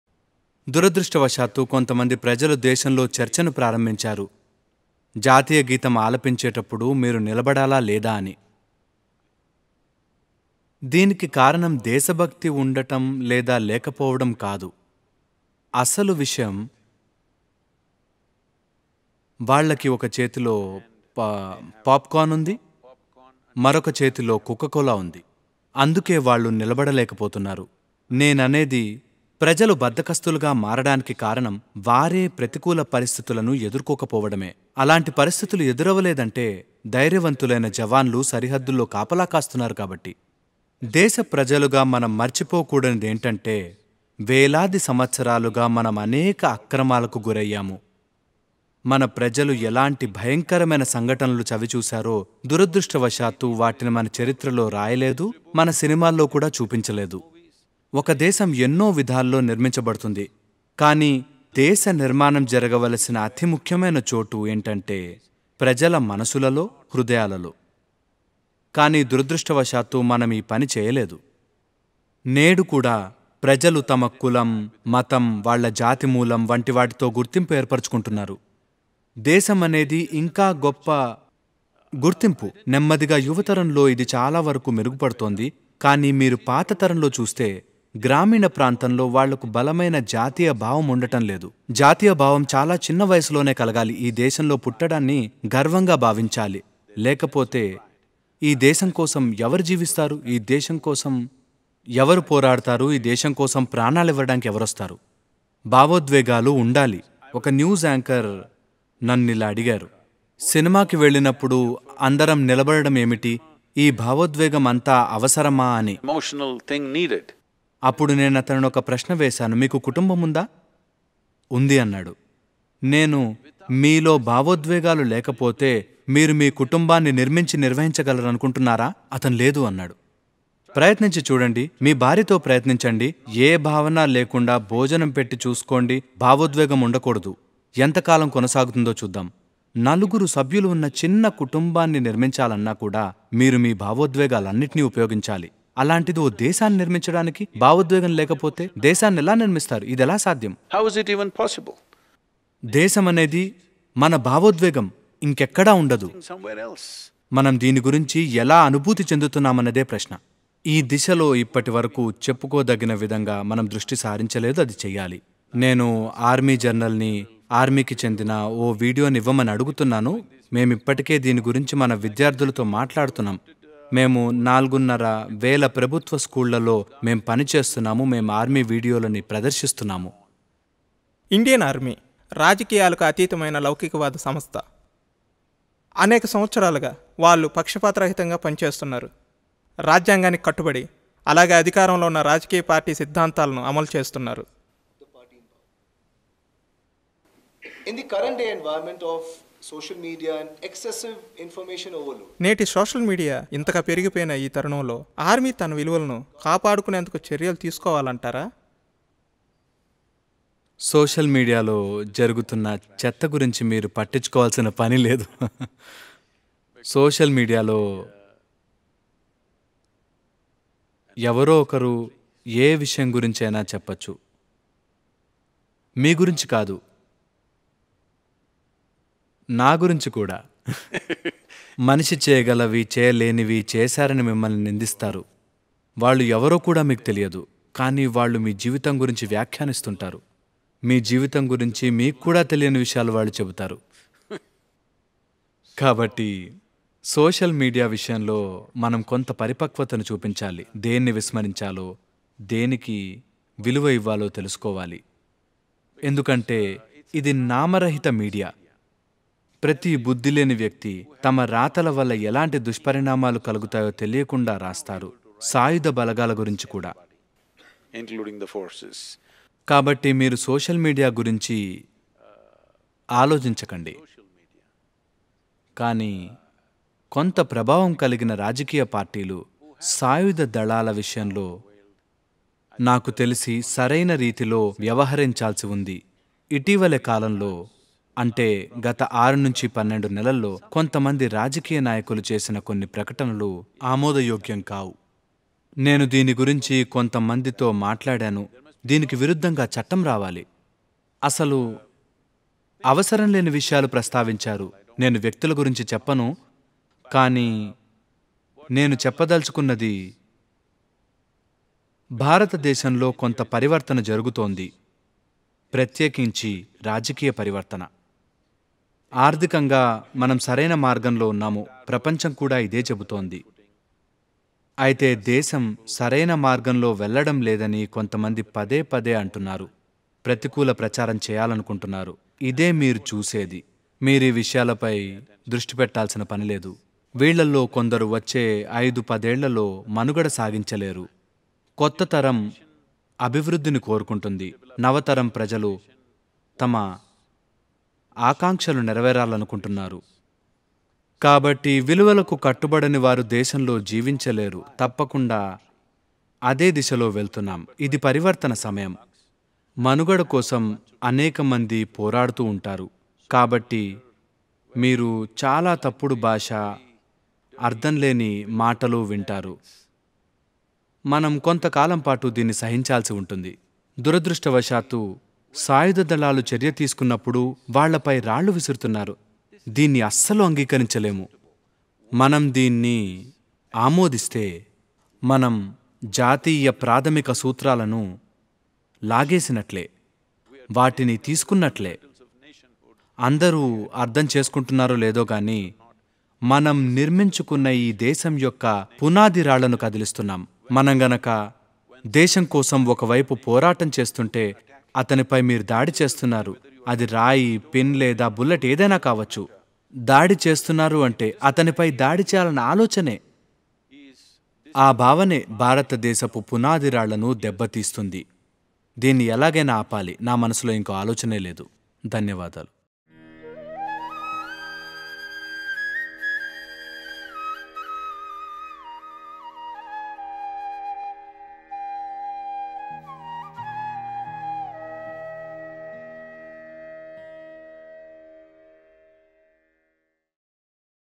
భారతదేశాన్ని ప్రమాదంలోకి పడేసే మూడు విషయాలు | Bharata Deshanni Pramadamlo Padese Moodu Vishayalu
లెహ్, లడఖ్ లో సద్గురు మన భారత సైన్యాధికారులతో సంభాషణ జరిపారు. ఈ సంభాషణలో భాగంగా మన దేశాన్ని పట్టి పీడించే, భారతీయతను దెబ్బతీసే విషయాల గురించి చర్చించారు.